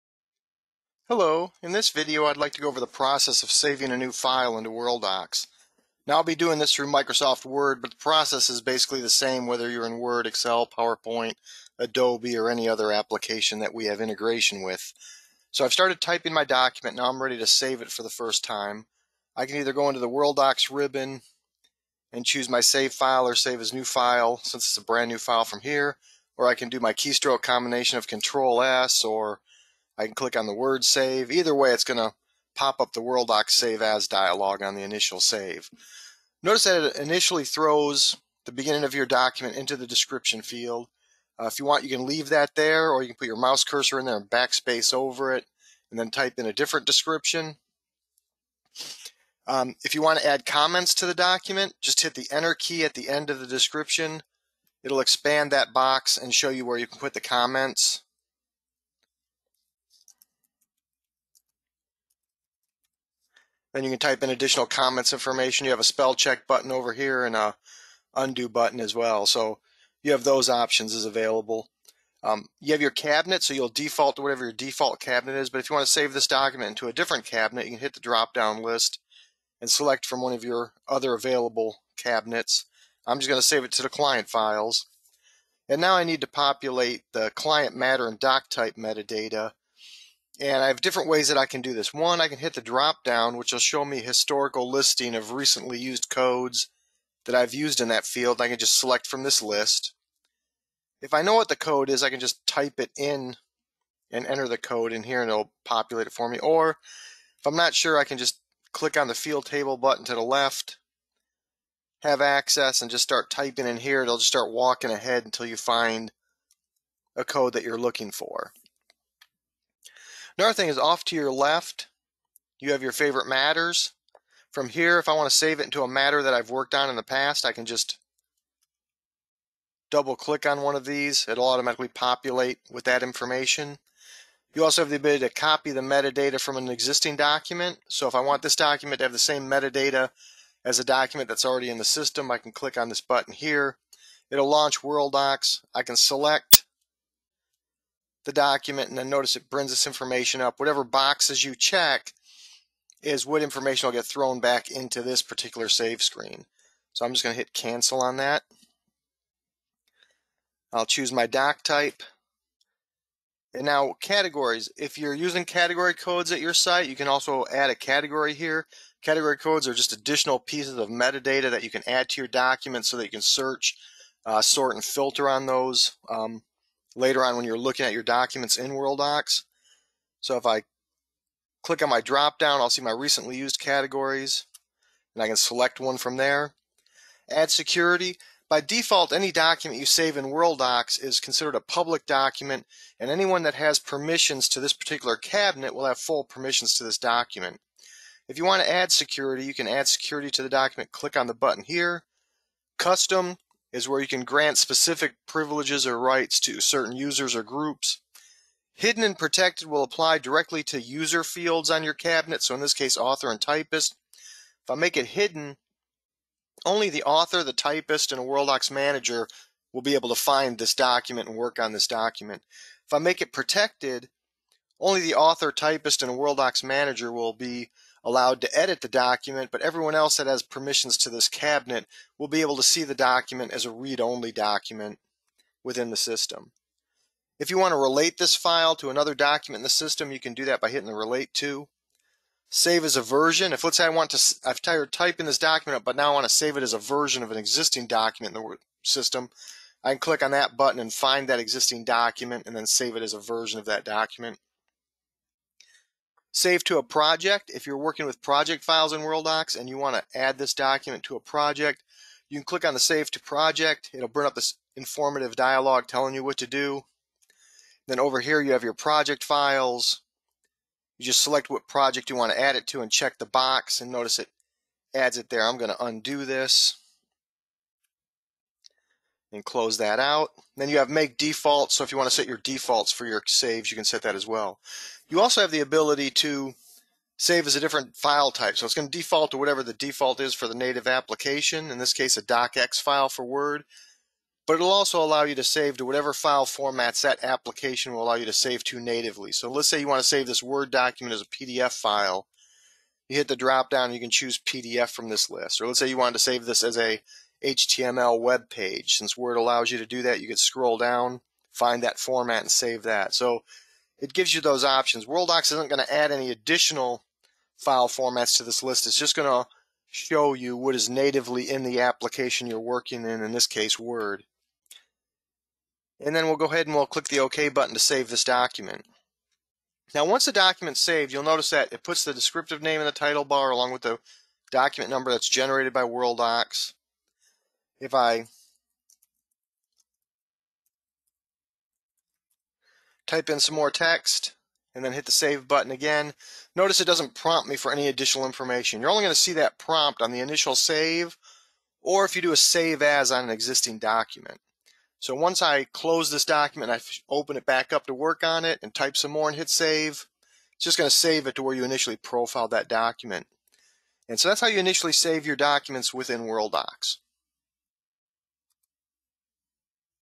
File Save
Saving a new Document in Worldox via Microsoft Word